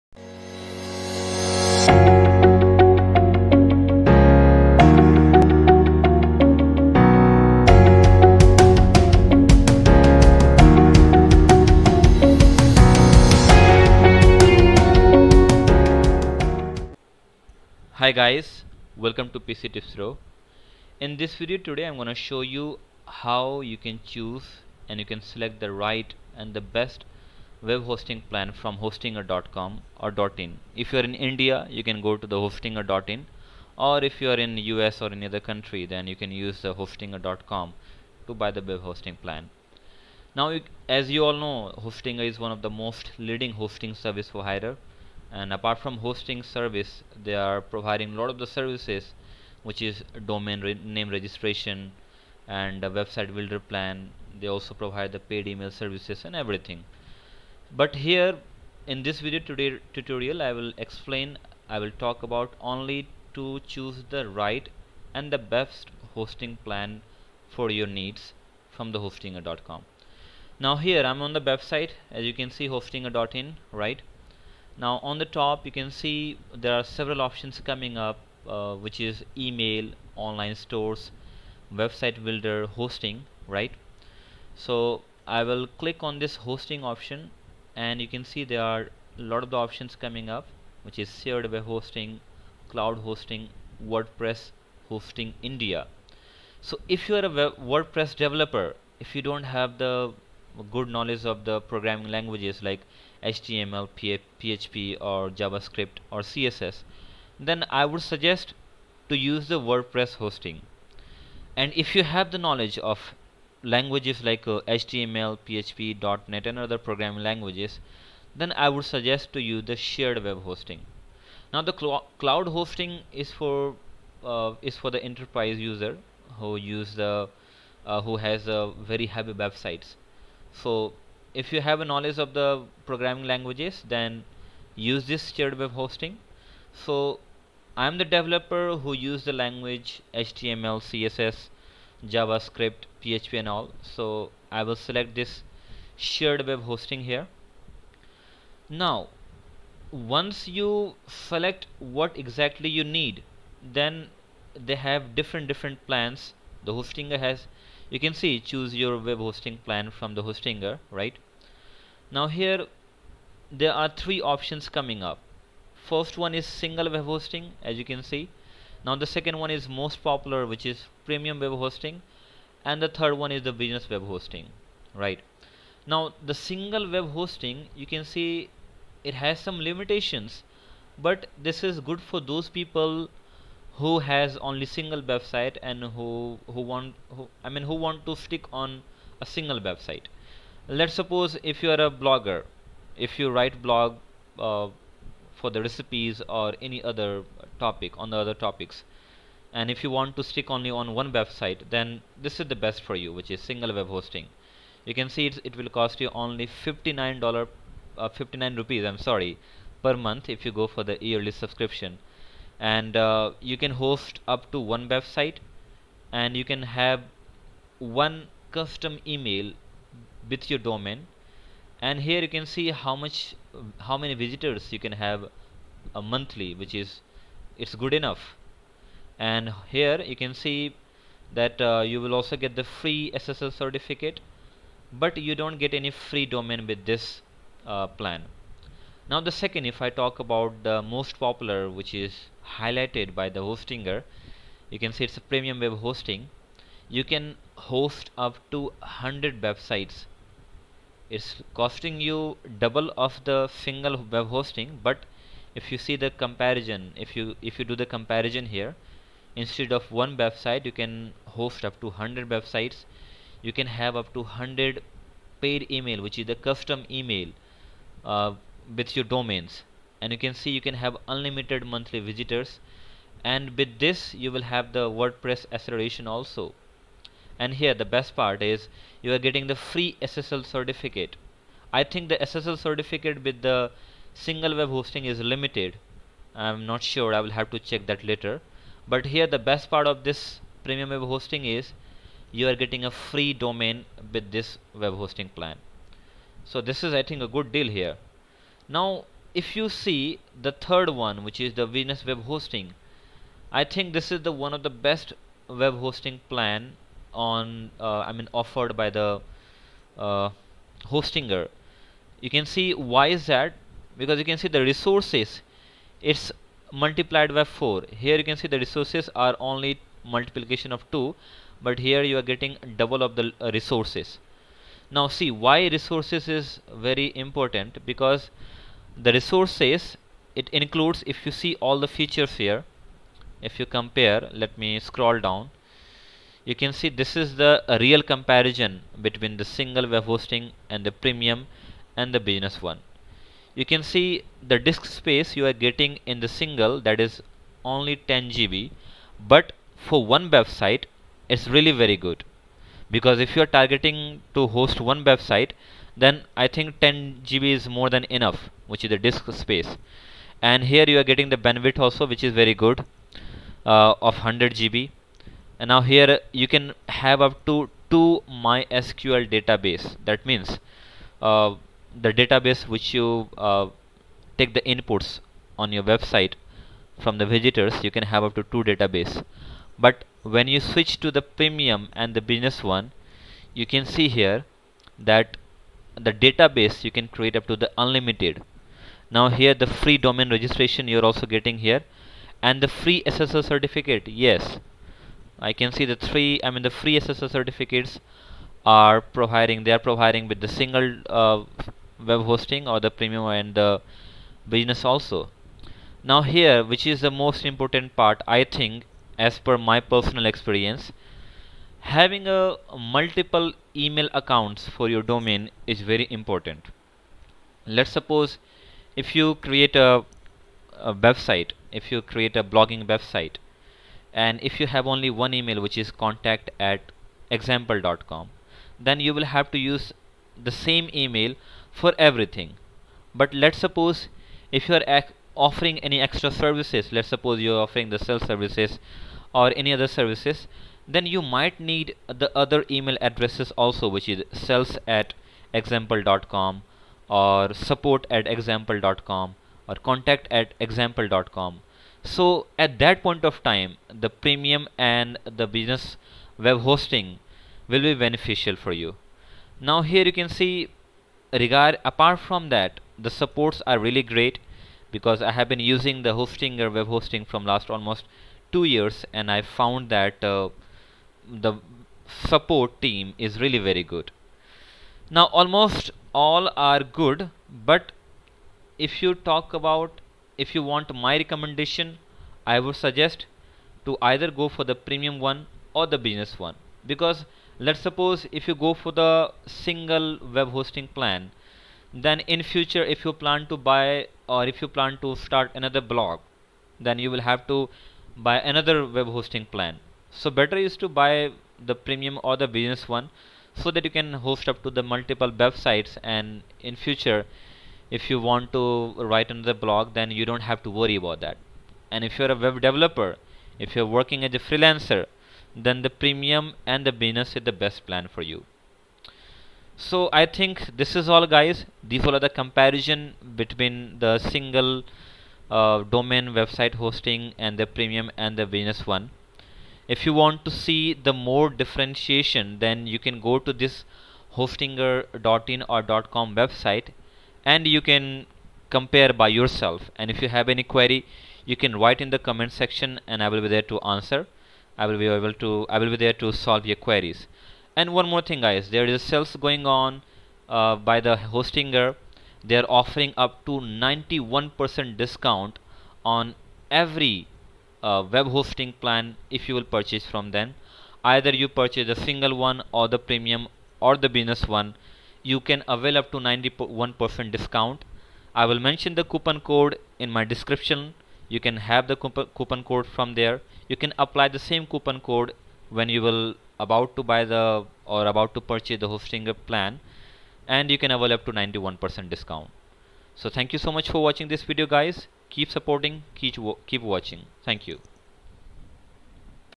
How to Choose Right Web Hosting Plan on Hostinger | Comparing Hostinger Website Hosting Services
Hostinger is offering various Web Hosting plans but choosing the right Web Hosting is very important.

In this video tutorial, I have explained how to compare and choose the right Website Hosting services on Hostinger.